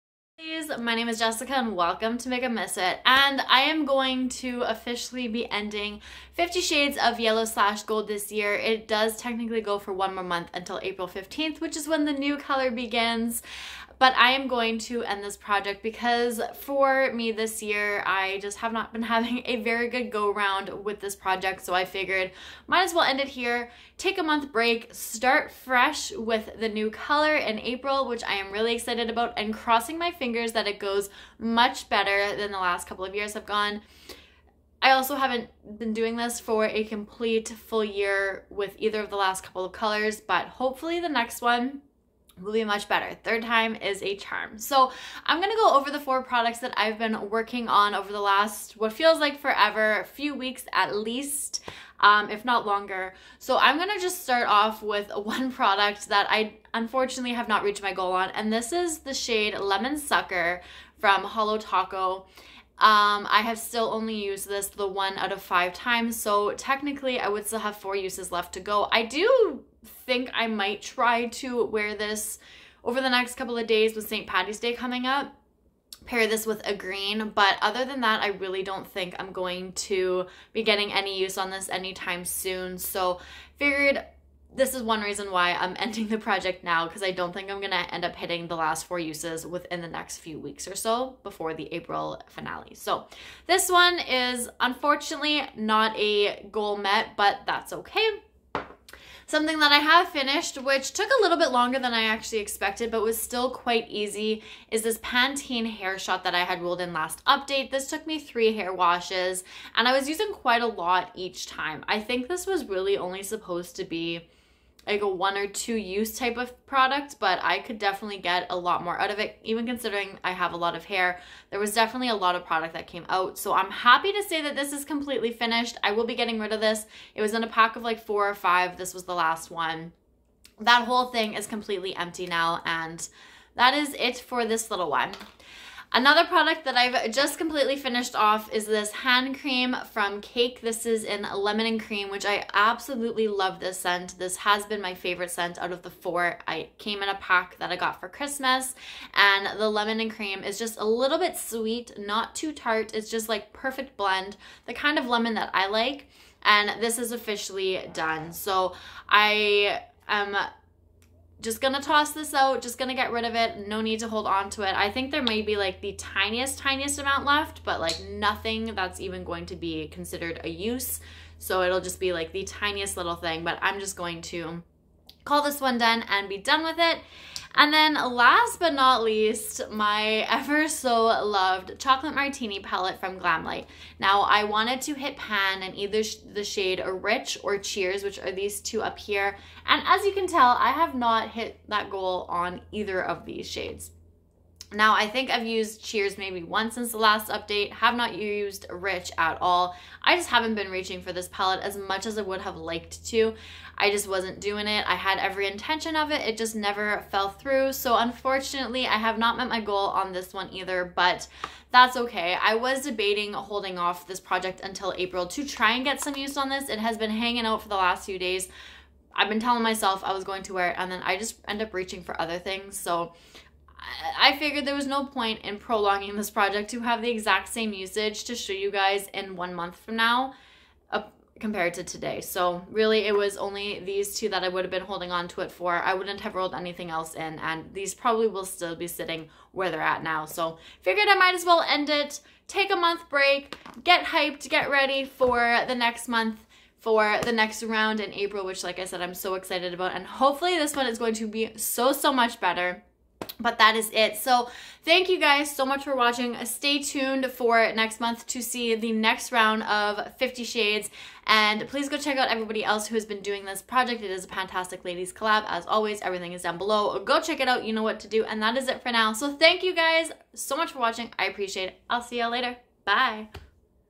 50shades of Yellow/Gold | finale
Hii lovelies!!

I have finally had  a chance to start this project, even though I have wanted to since before it began in April.

I will only be working on a few products at a time, 3-5, and taking my time. I just want to see how many yellow/gold products, or products with yellow/gold on them, i can finish before this ends in April.

f o c u s |
glamlite rich/cheers
cake lemon & cream hand cream

o u t |
holo taco lemon spritzer
pantene hair shot
bbw body cream
bbw marshmallow fireside candle
davds tea advent calendar (1 calendar, 20 teas)
bbw champagne toast spray
abh gold eyeshadow
becca highlighters